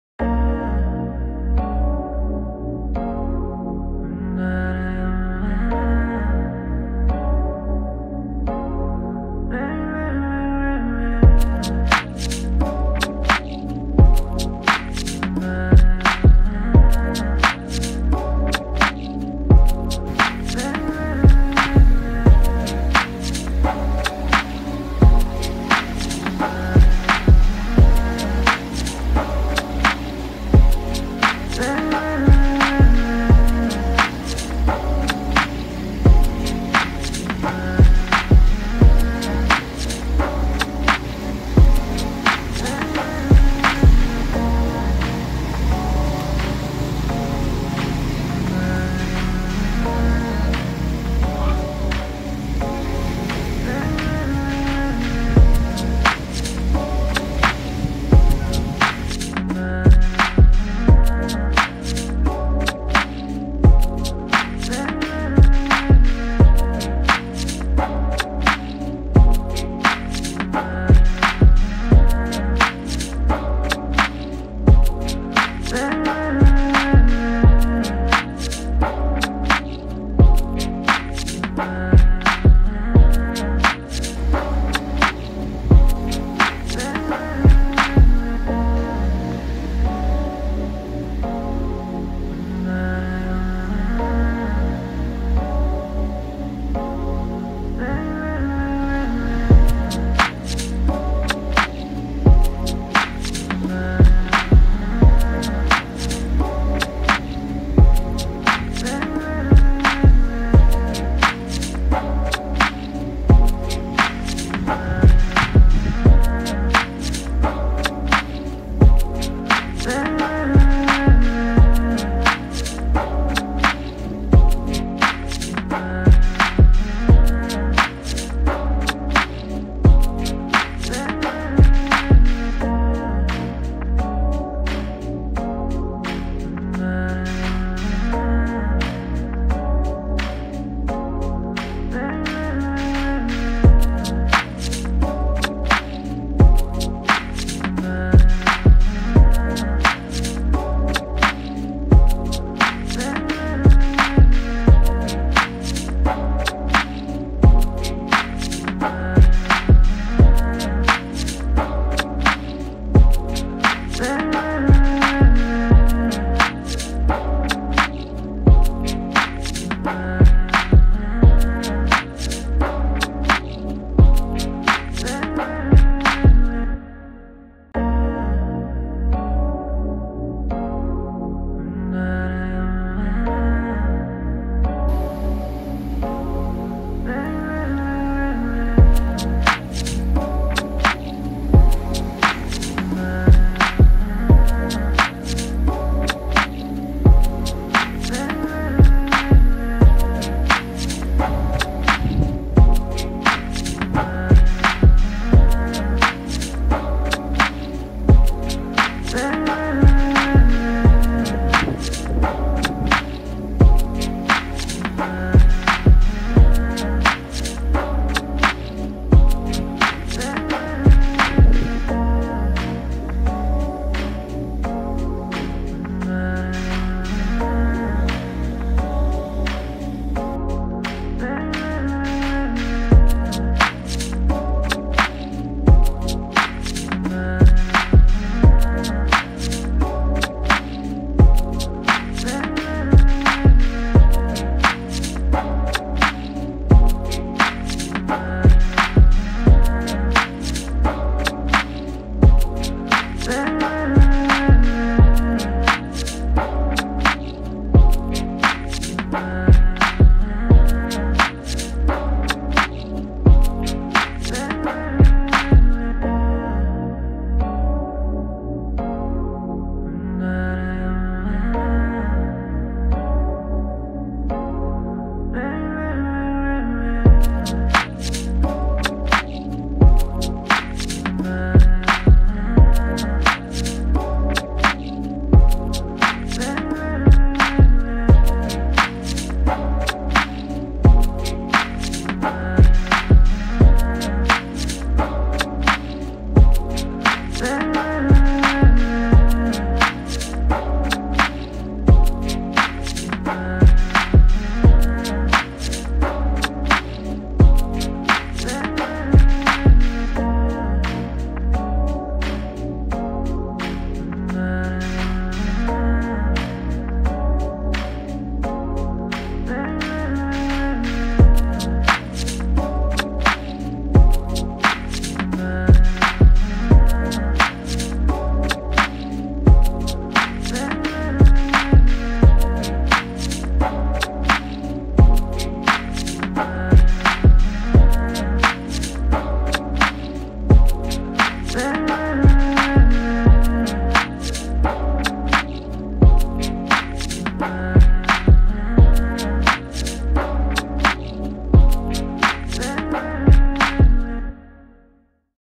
Thailand, Hat Chao Lao Beach Day 2, Chanthaburi
Chao Lao Beach, on the Gulf of Thailand, is so gorgeous - very relaxed and peaceful.  The tide goes out far, so wait for it to come in before going swimming - The ocean is calm and the beach is a great place for families or groups wishing to relax by the Gulf of Thailand.